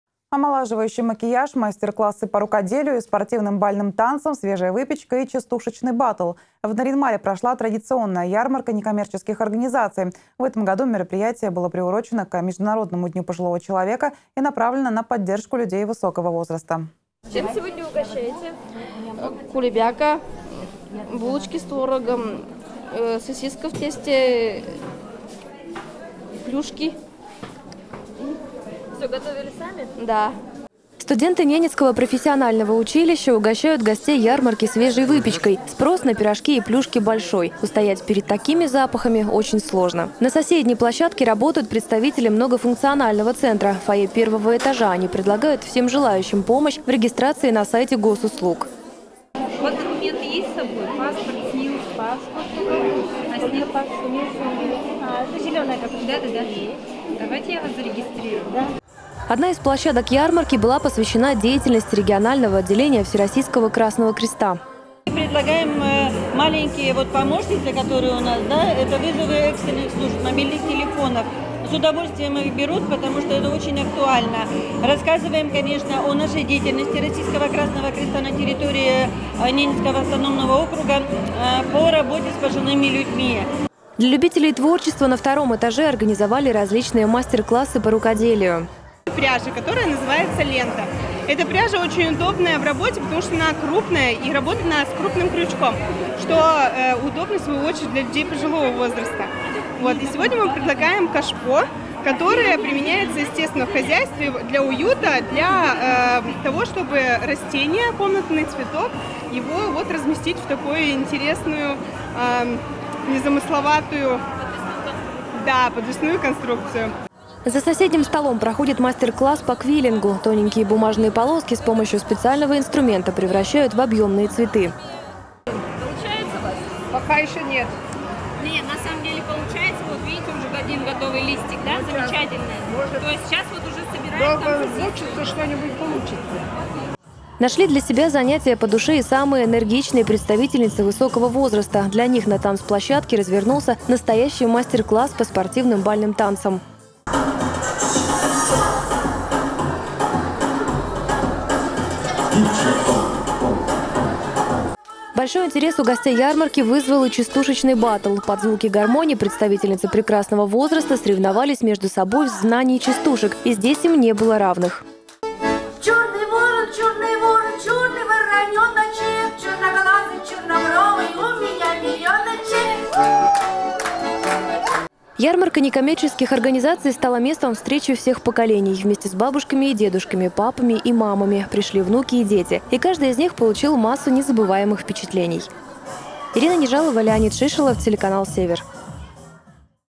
Ярмарку НКО в Нарьян-Маре приурочили к Дню пожилого человека
Омолаживающий макияж, мастер-классы по рукоделию и спортивным бальным танцам, свежая выпечка и частушечный батл. В Нарьян-Маре прошла традиционная ярмарка некоммерческих организаций. В этом году мероприятие было приурочено к Международному дню пожилого человека и направлено на поддержку людей высокого возраста.